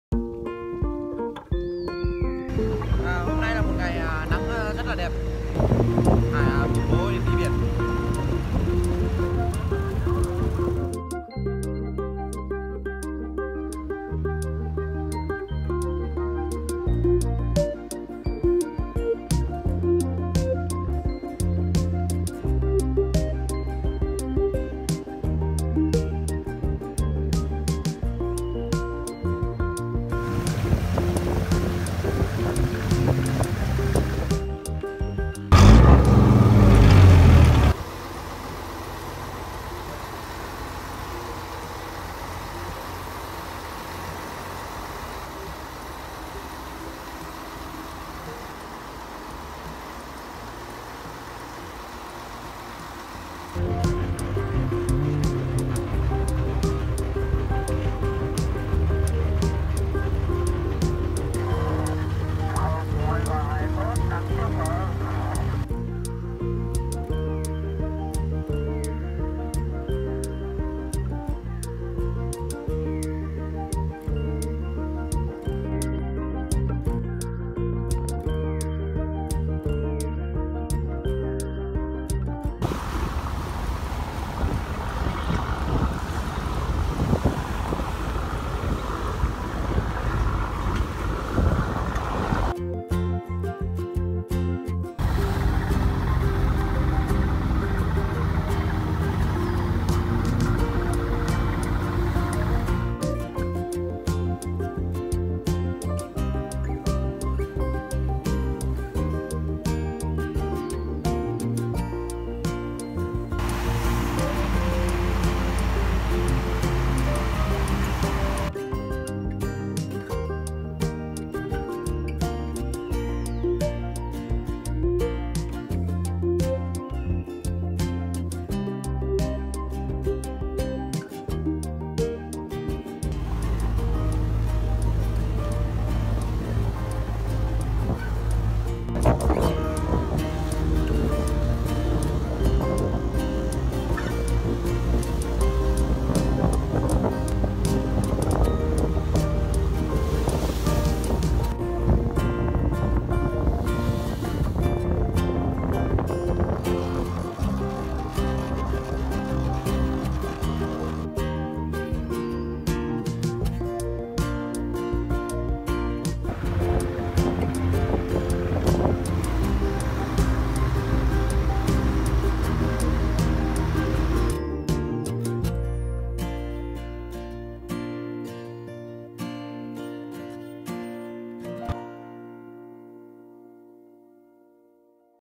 Giã Cào Diễn Bích - Diễn Châu, Nghệ An
Chuyến đi biển tại Diễn Bích.
Video do Phạm Vĩnh Hải thực hiện.
Đây là kênh đầy đủ thông tin về huyện Diễn Châu Nghệ An.

Cảm ơn các bạn đã ủng hộ